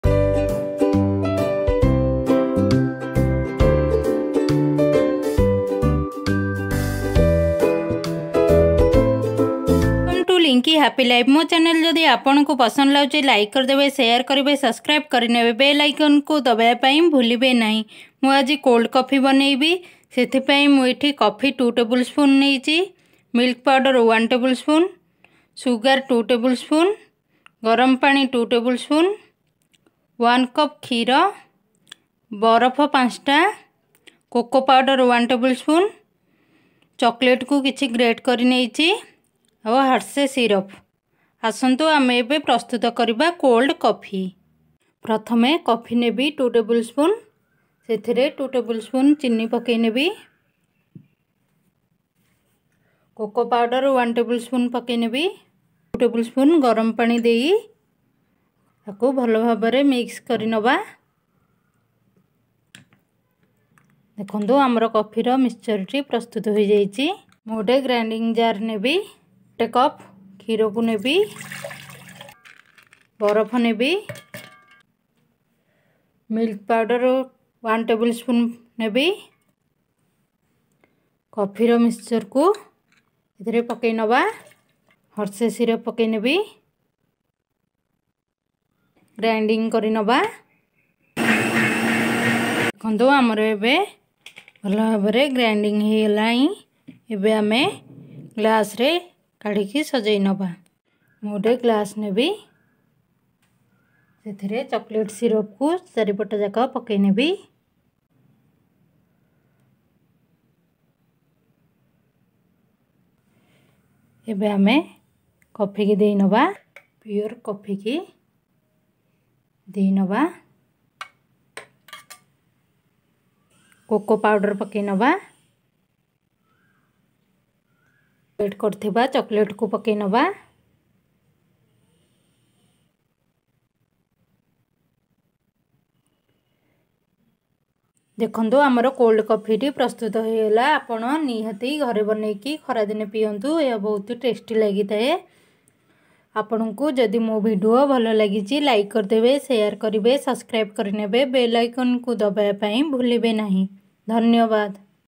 କୋଲ୍ଡ କଫି। Cold Coffee🥤 Recipe without ice cream at home।By Linky happy life.
Cold coffee is a refreshing and tasty drink. Prepared in a simple way without icecream. Enjoy it at home. please follow my video.
ingredients-- Coffee powder-2tbspoon, Milk-1cup,  Sugar-2tbspoon, Coaco powder-1tbspoon, Hot water-2t spoon, Milk powder-1tbsp, ice cube-5, Harshey syrup, Greated dark chocolate.